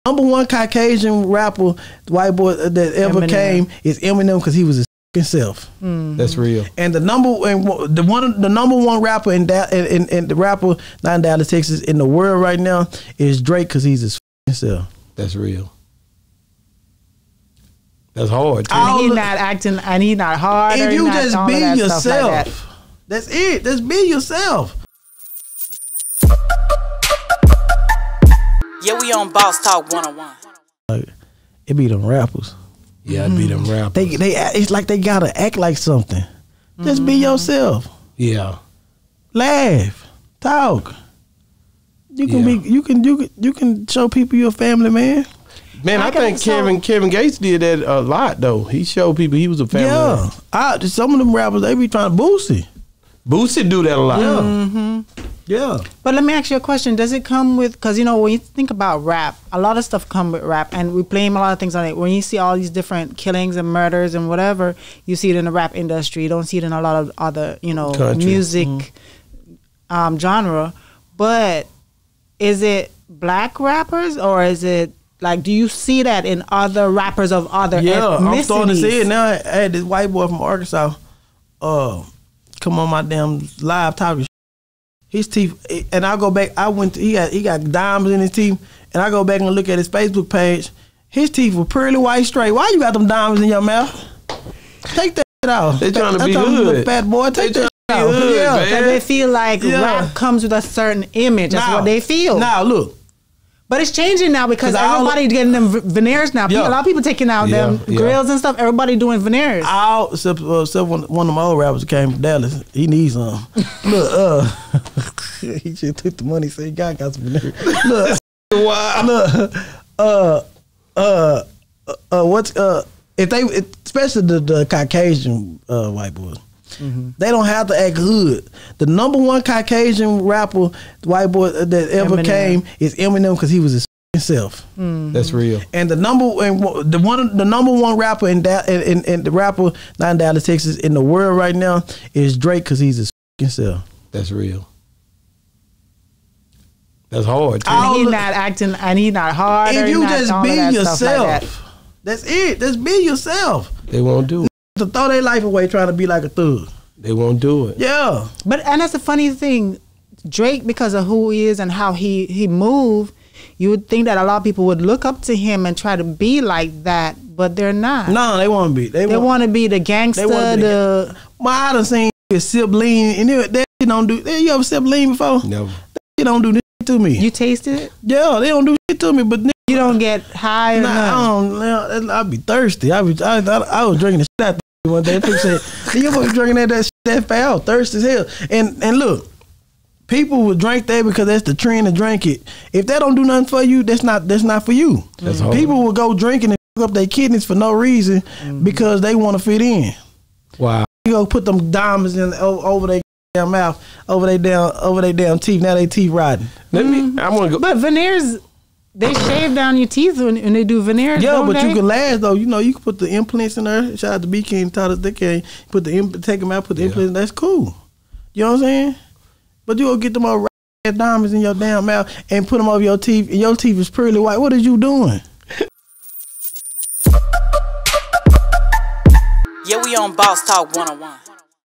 Rainwater on Drake & Eminem Just who they Are Rappers Need to JUST BE THEMSELVES! +More (Part 13)
Rainwater on Drake & Eminem Just who they Are Rappers Need to JUST BE THEMSELVES!